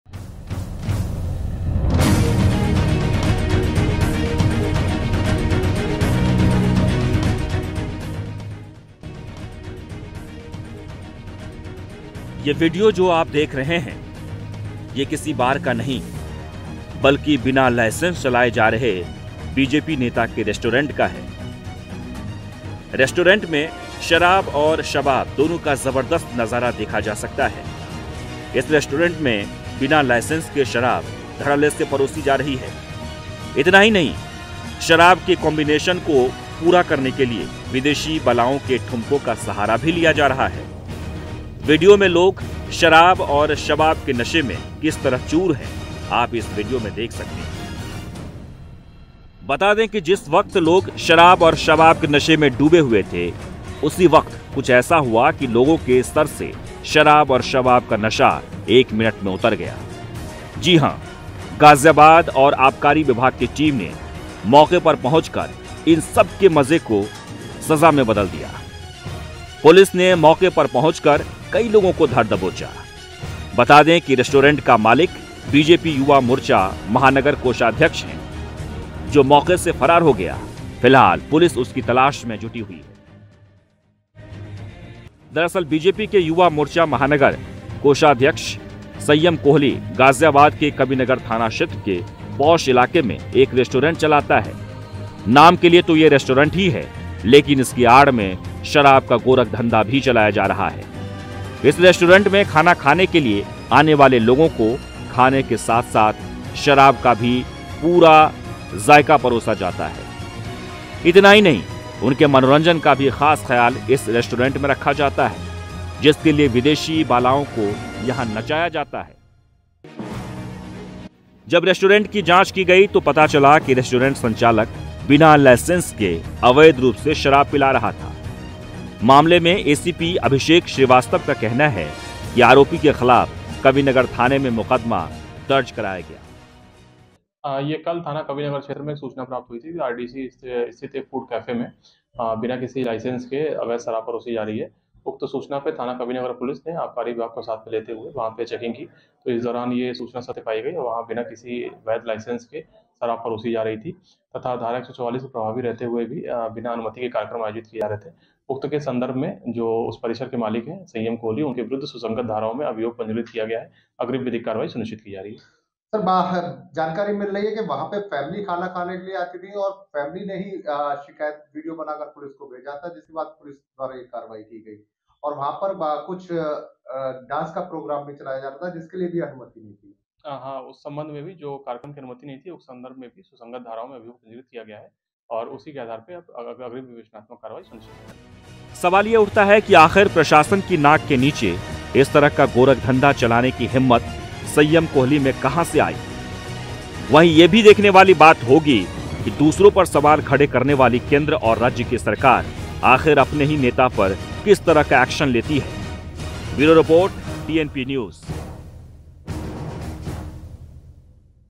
Ghaziabad:बीजेपी नेता के रेस्टोरेंट में छलक रहे थे जाम,विदेशी बारबालाएं लगा रहीं थी ठुमके
Ghaziabad:बीजेपी नेता के रेस्टोरेंट में छलक रहे थे जाम,विदेशी बारबालाएं लगा रहीं थी ठुमके || TNP NEWS ||